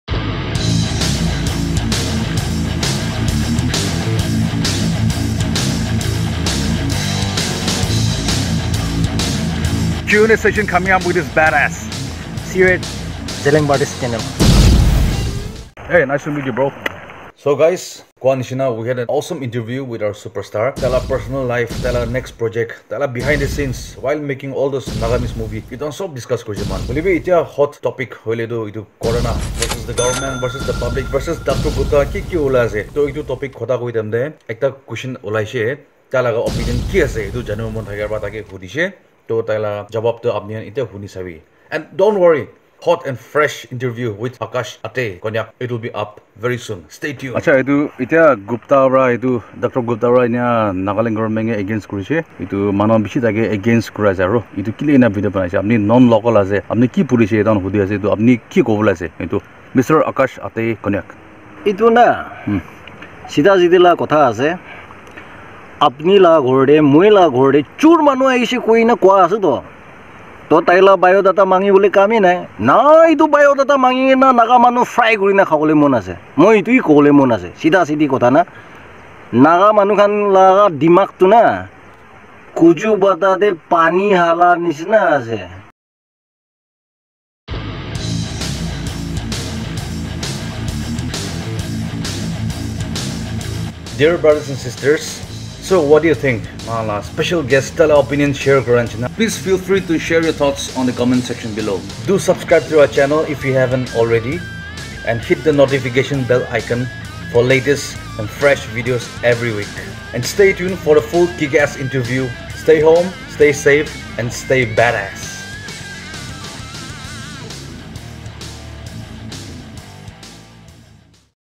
DR PANKAJ GUPTA VS RIO | TRUTH REVEALED | NAGAMESE SUPERSTAR SPEAKS OUT | ZELIANG BADASS
Q AND A WITH OUR SUPERSTAR AKASH ATEI KONYAK ..ACTOR , PRODUCER AND DIRECTOR OF NAGA FILM INDUSTRY .